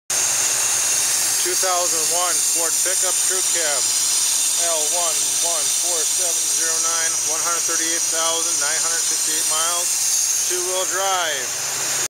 l114709 2001 Ford F150 crewcab 5.4 automatic 4wd 138968 miles Elmer's Auto Inc.
Parting out 2001 Ford F150 Crewcab 4wd 5.4 automatic with 138968 miles. Stock # l114709.
Need parts, call us!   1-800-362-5 O O 4  ext 221
Eric